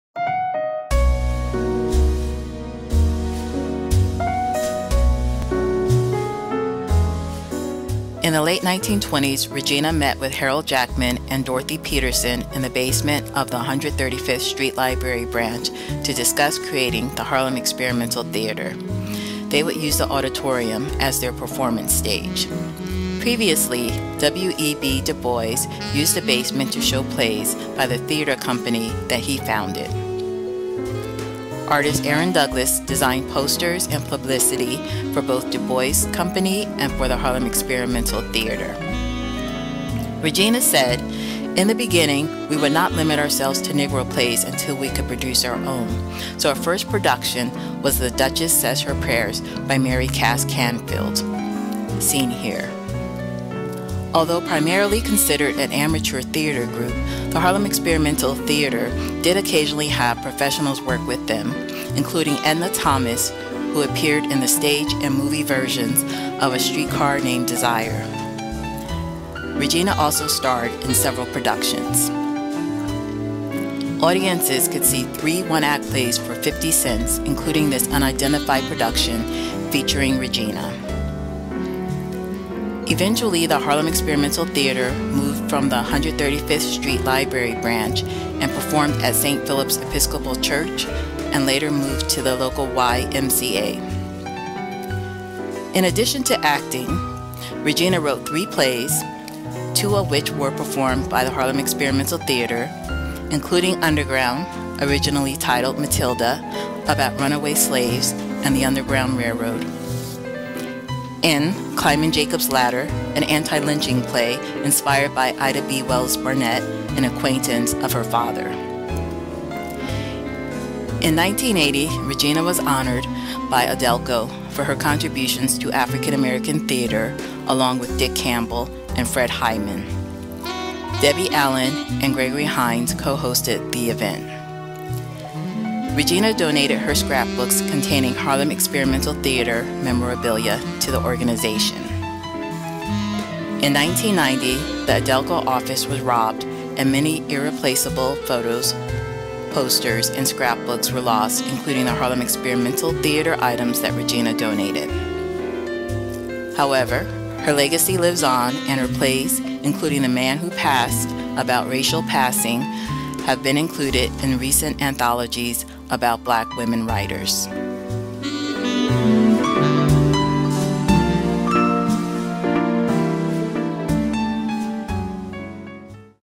Chapter Five - The Harlem Experimental Theatre
This is about librarian Regina Andrews' work with the Harlem Experimental Theatre.